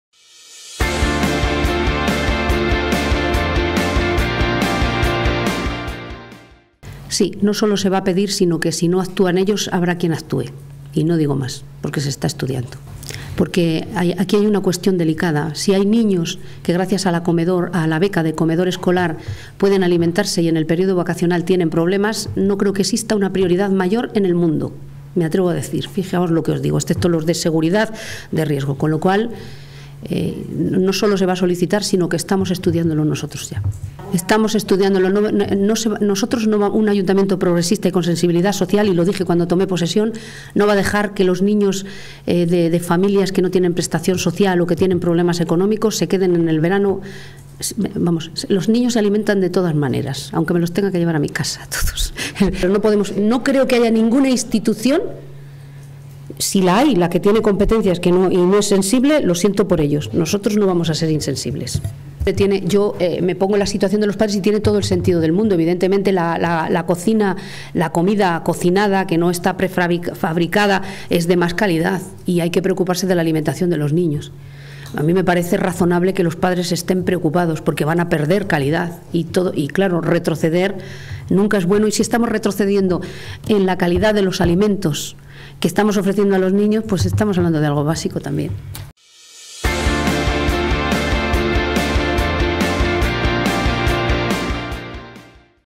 El Ayuntamiento pondrá en marcha un programa de alimentación infantil para familias vulnerables
El Ayuntamiento de Segovia incorpora un nuevo programa a los ya existentes para ayudar a las familias segovianas en situación de vulnerabilidad.